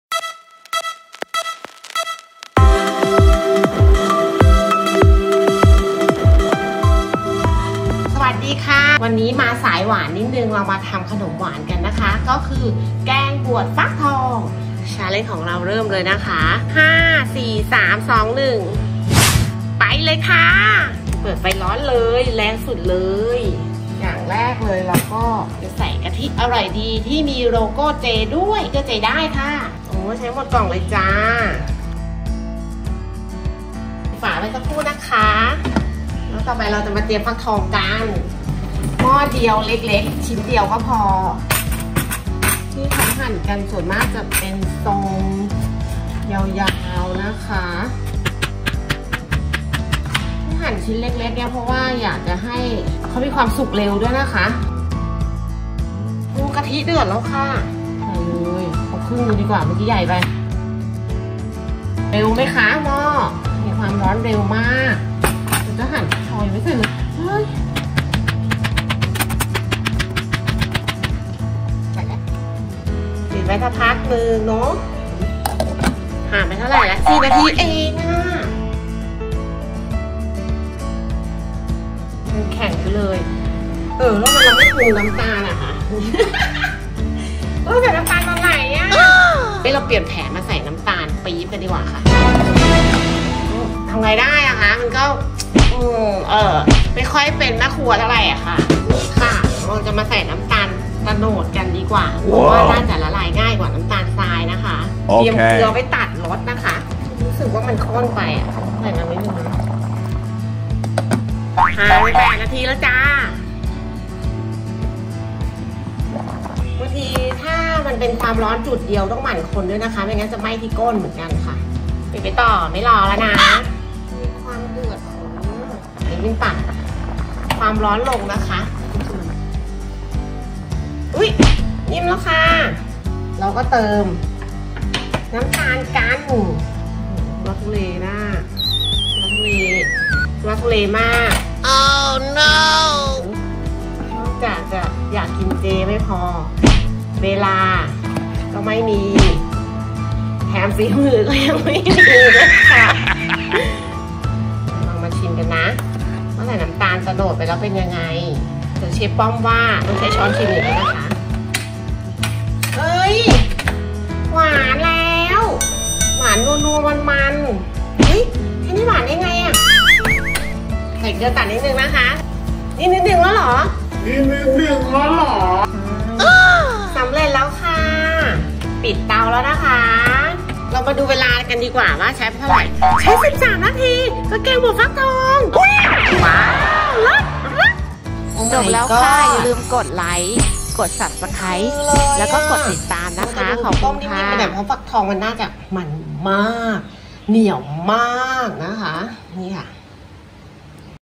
ง่ายๆและรวดเร็ว
วันนี้เป็นอาหารเจเมนูที่ 7 นะค่ะ
สาวออฟฟิศจะมาทำอาหารเจในออฟฟิศ ด้วยหม้อไฟฟ้าอเนกประสงค์
 (เมนูนี้ใช้เวลารวม 13 นาทีกว่าๆ)
แกงบวดฟักทองกะทิกล่อง อร่อยดี ขั้นตอนในการทำอาจจะไม่เป๊ะ
แต่ท้ายที่สุดแล้ว รสชาติออกมาเป๊ะนะค่าาาา

อาหารเจ เมนูเจ ไม่ยากเลยจ้าา
อิ่มบุญ อิ่มใจ ไปพร้อมกัน สาธุ.